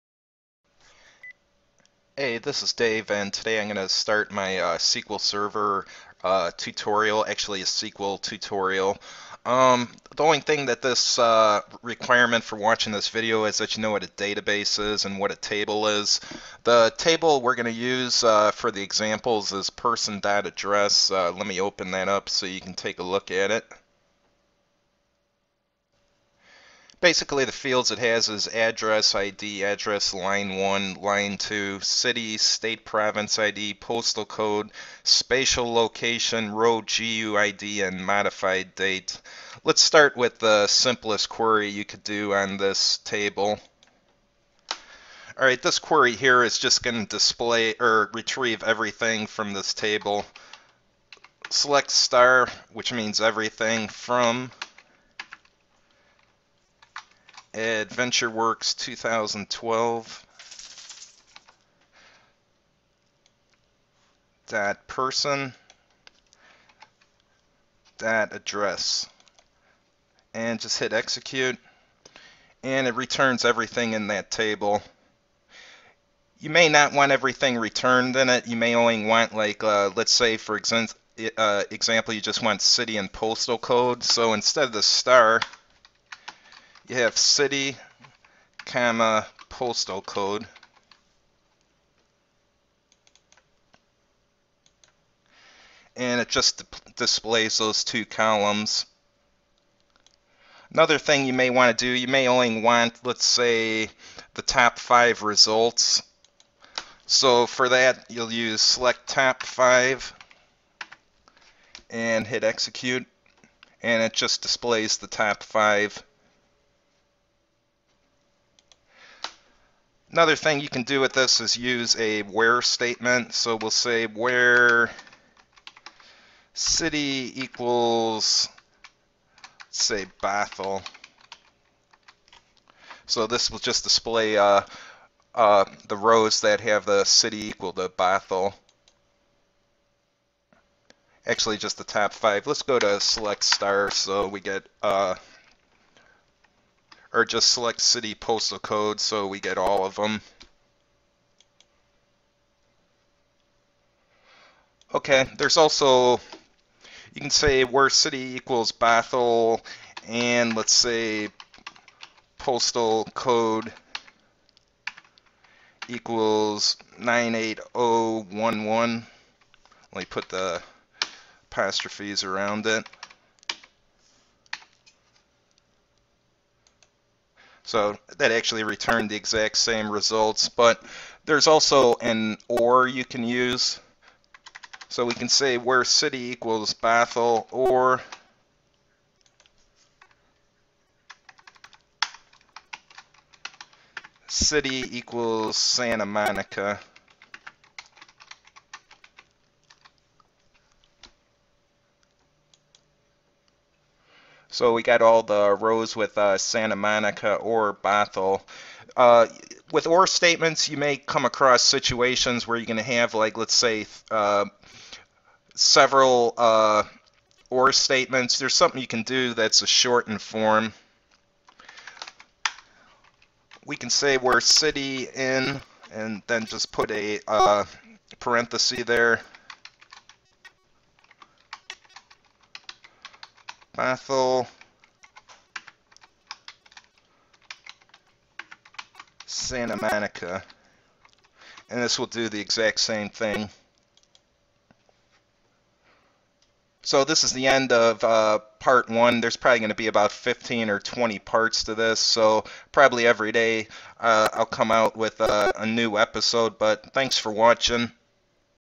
SQL Tutorial #1 Select, *, TOP, WHERE, IN, AND, OR
The first SQL Tutorial episode.  The select statement, *, TOP, WHERE, IN, AND, OR are covered.